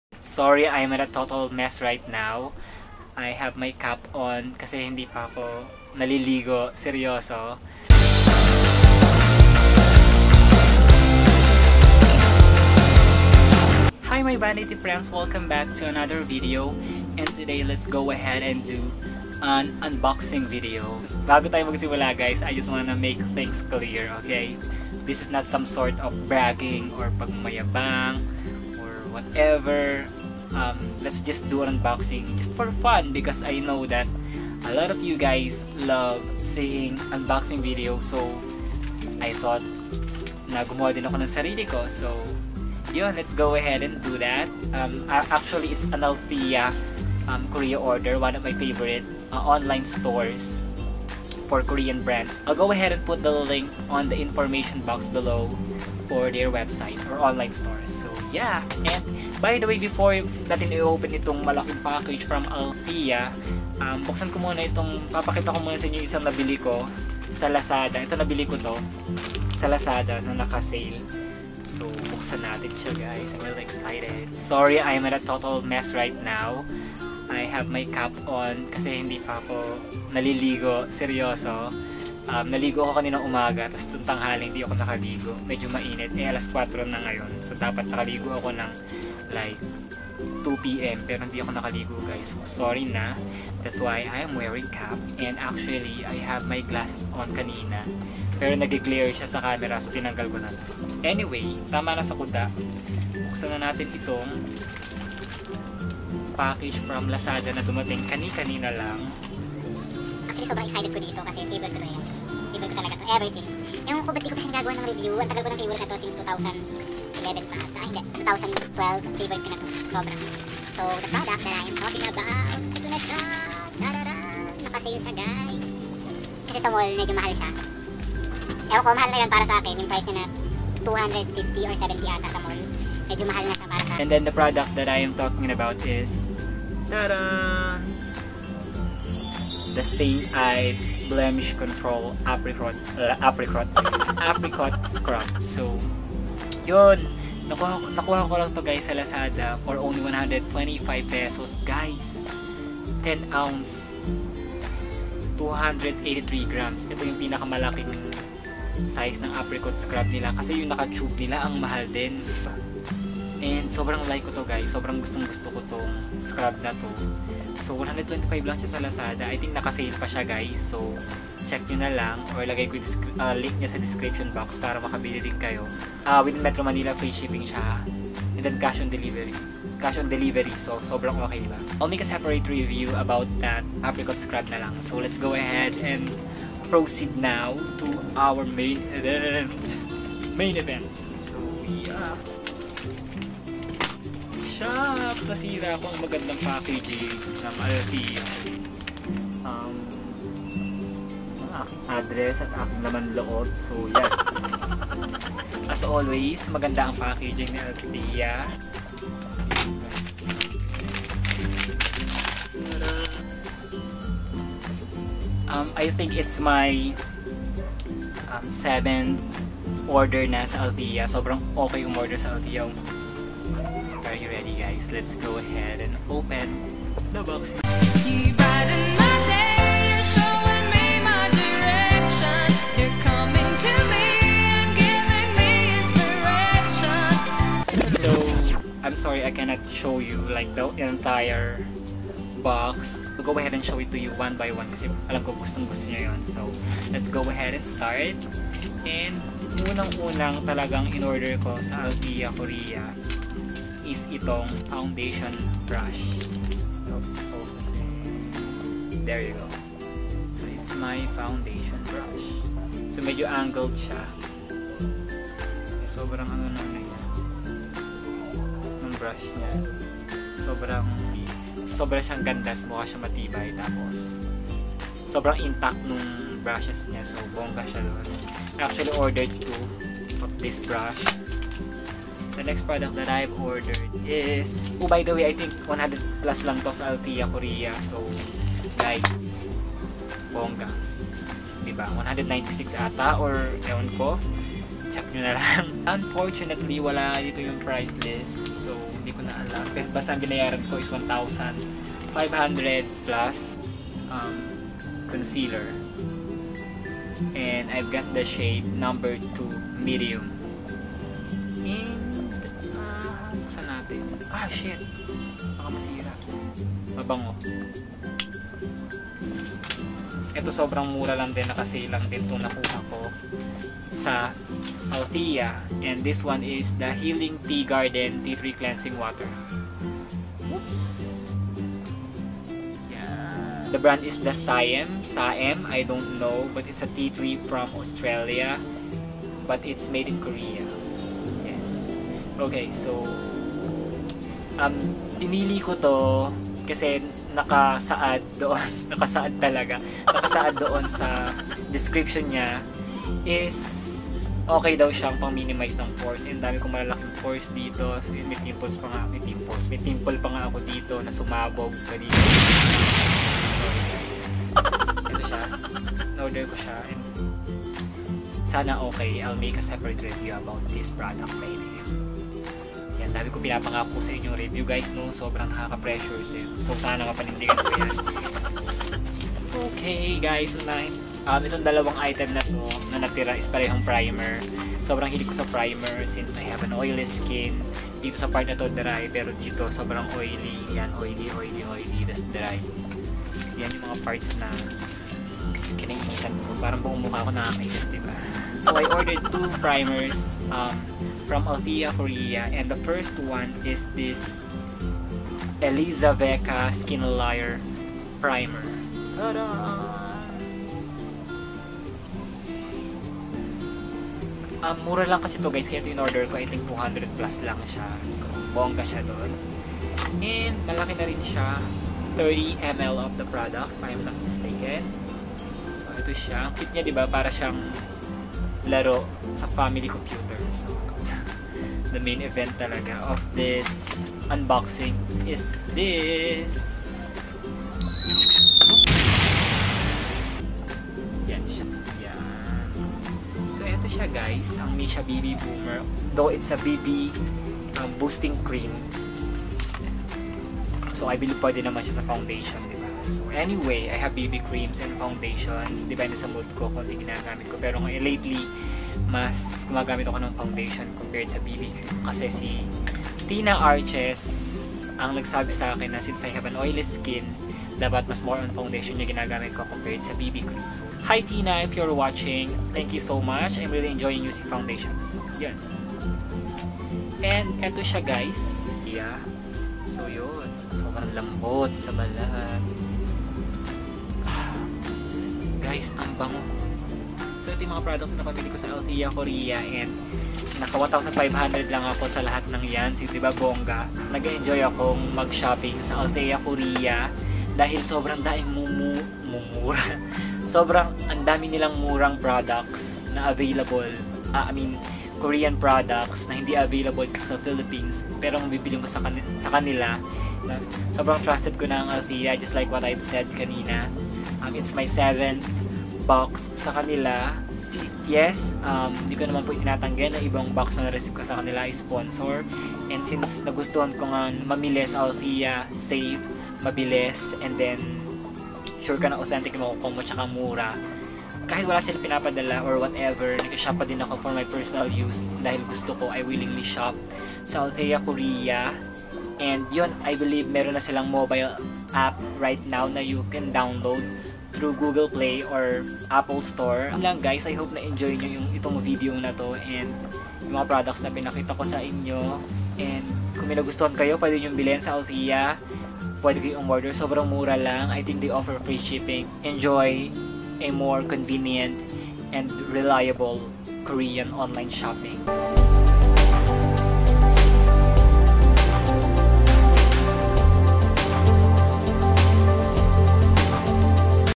Althea Korea Unboxing | Best deals
Hello guys. Here's another tipid shopping Unboxing for you.